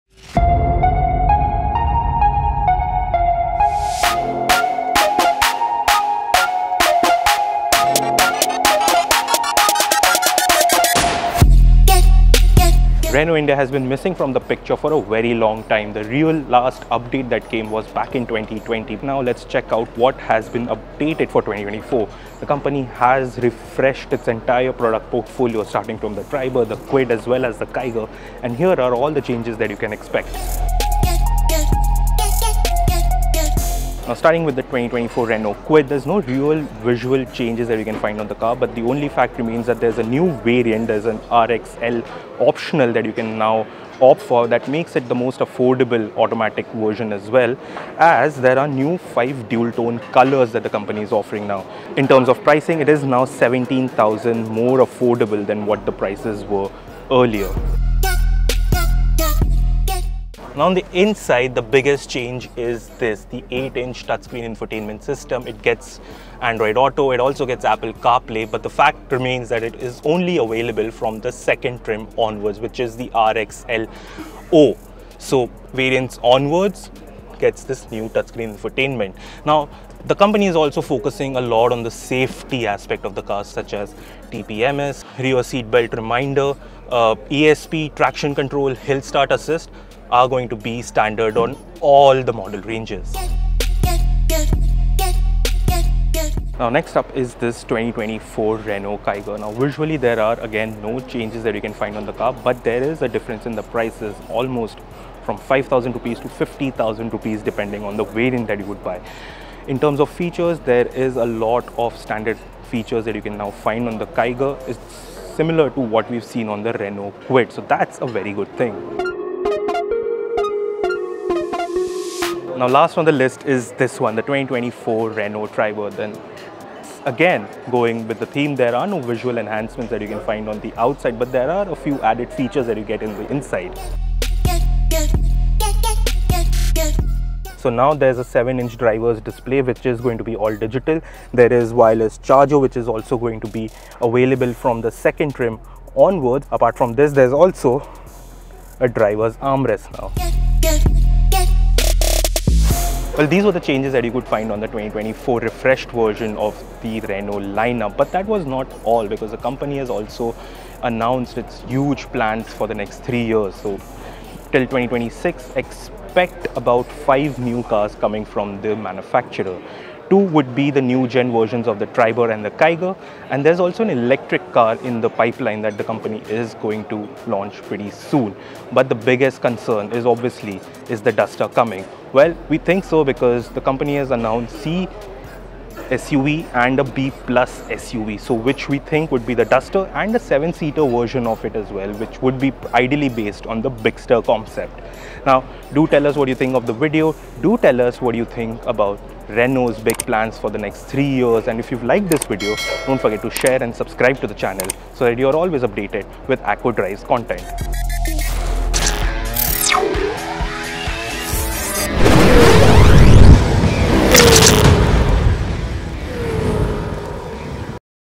2024 Renault Kwid, Kiger, Triber Launched
Renault India has updated it's entire product portfolio for 2024, including refreshed versions of the Kwid, Kiger and Triber. There are no visual enhancements but new features get added on all the models updated with new features list. Take a look at what's new from Renault.

Cameraperson: Jitender
Editor: Hari Kumar